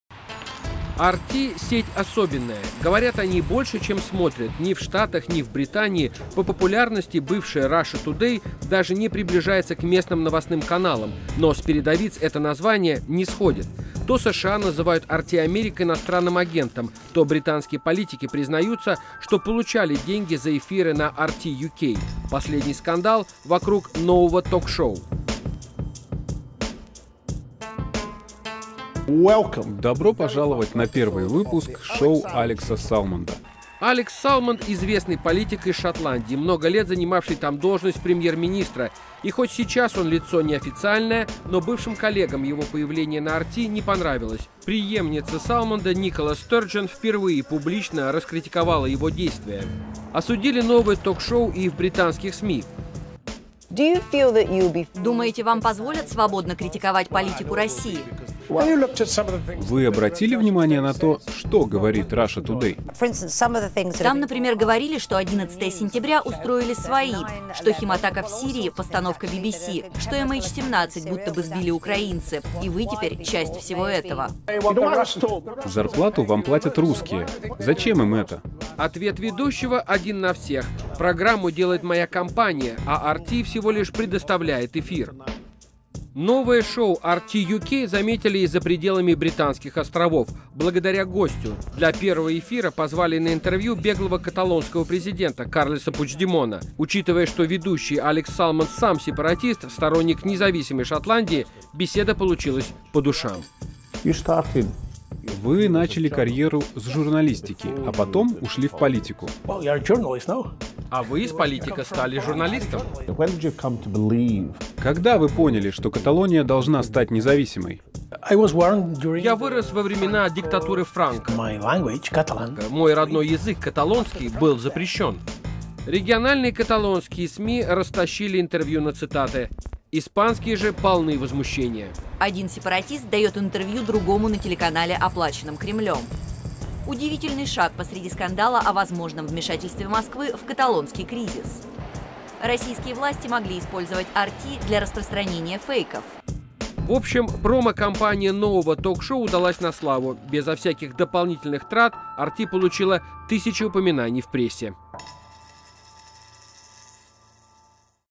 RT спокойно работает и выпускает новые шоу
Политики, упорно цепляющиеся за должности и уходящую популярность, - зрелище любопытное. Настолько, что в новом ток-шоу на британской версии RT - участвуют сразу двое таких персонажей. Бывший премьер Шотландии берет интервью у беглого президента Каталонии. И все это посреди скандала об “иностранных агентах”.
В Штатах посчитали, что RT America может быть инструментом влияния и пропаганды и потребовали для канала специального статуса. Россия ответила, как считает, симметрично. На этой неделе закон о СМИ-иностранных агентах успешно прошел голосование в Совете Федерации. И теперь уйдет на подпись президенту. Закон может ограничить работу любых СМИ, которые Минюст внесет в “черный список”. Это касается и нашего телеканала.